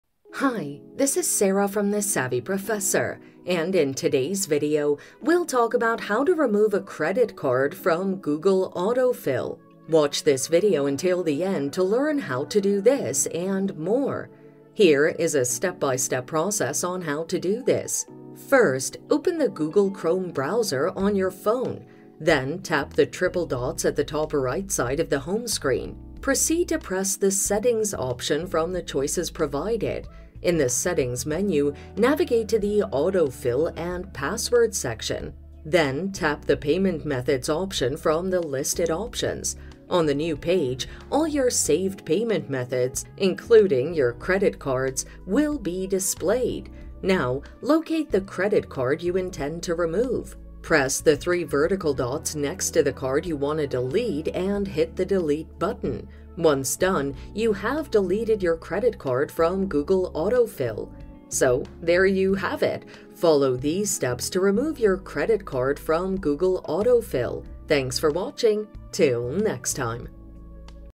How To Remove Credit Card From Google Autofill (How Do You Delete Saved Credit Cards On Google?)
How To Remove Credit Card From Google Autofill (How Do You Delete Saved Credit Cards On Google?). In this video tutorial I will show you how to remove credit card from Google autofill.

Here is a step-by-step process on how to do this:
• First, open the Google Chrome browser on your phone.
• Then, tap the triple dots at the top-right side of the home screen.
• Proceed to press the Settings option from the choices provided.
• In the settings menu, navigate to the Autofill and Passwords section.
• Then, tap the Payment Methods option from the listed options.
• On the new page, all your saved payment methods, including your credit cards, will be displayed.
• Now, locate the credit card you intend to remove.
• Press the three vertical dots next to the card you want to delete and hit the delete button.
• Once done, you will have deleted your credit card from Google autofill.